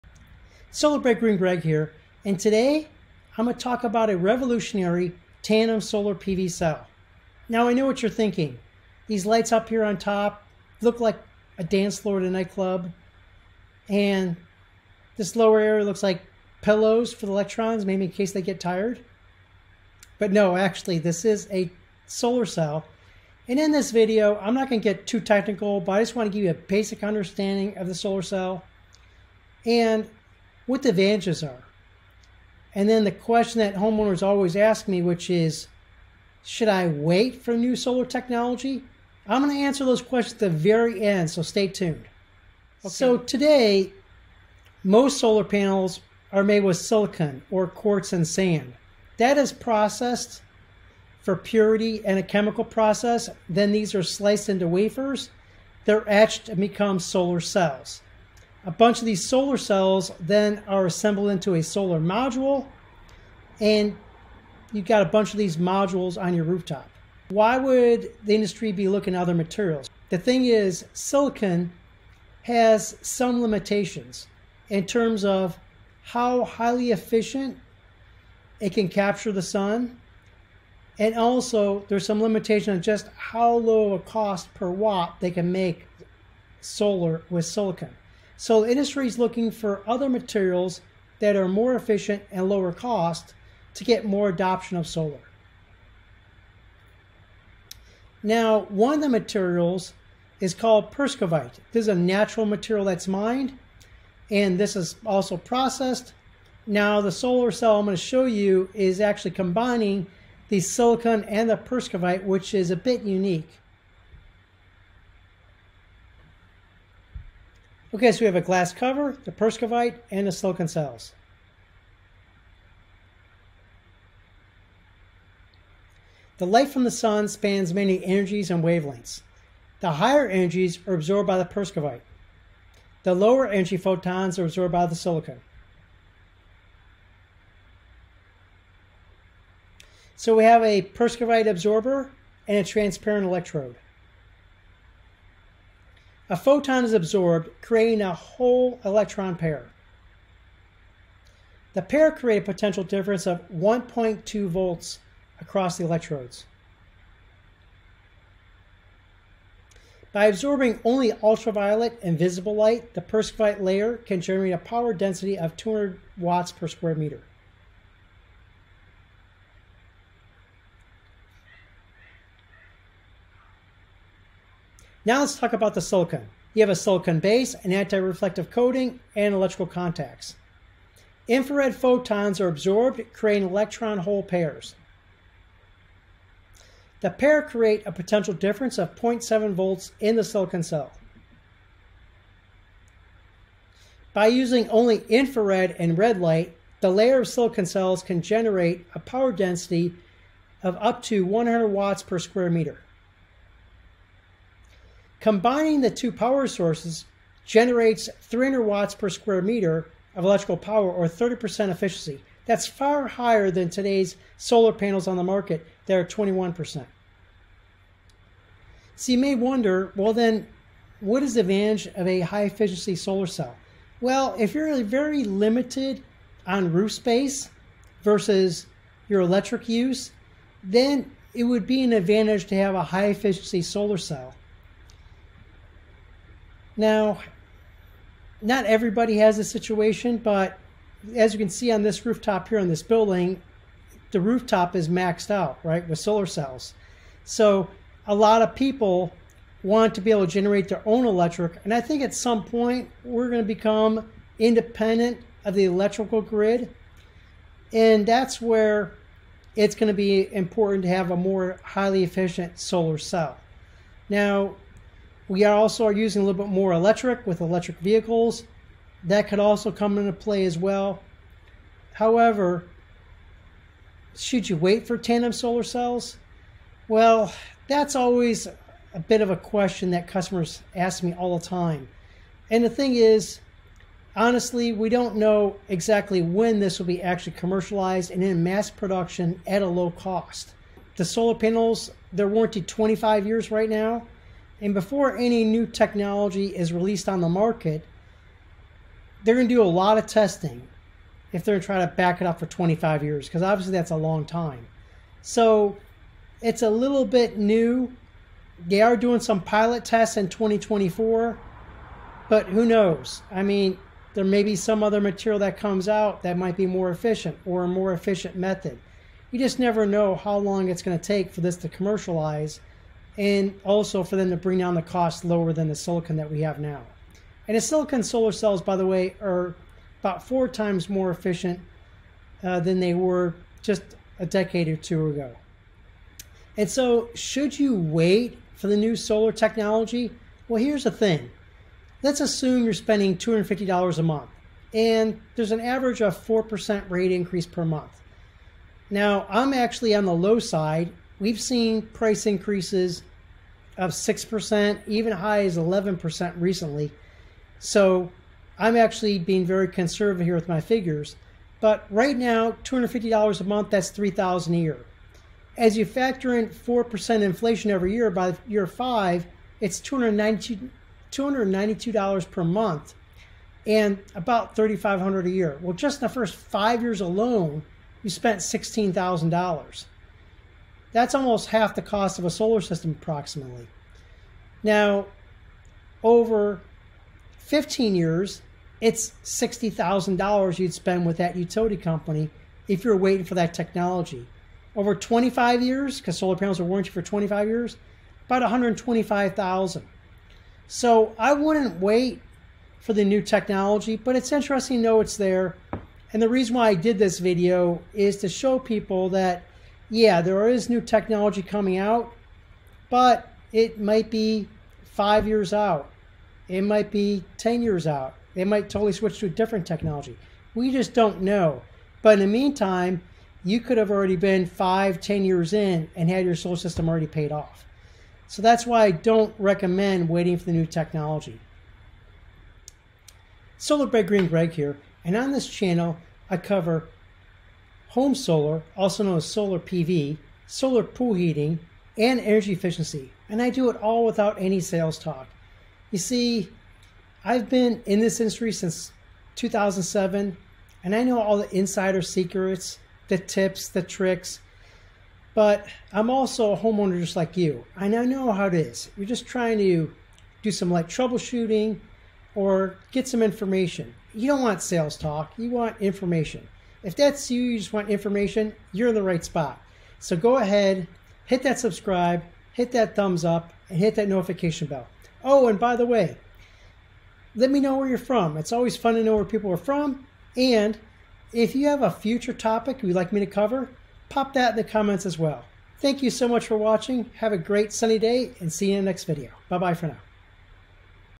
The Future of Solar POWER Should You Wait for New Solar Tech?
New Solar Power technology, Persovskites... increases solar panels efficiency. Should you wait to buy solar panels?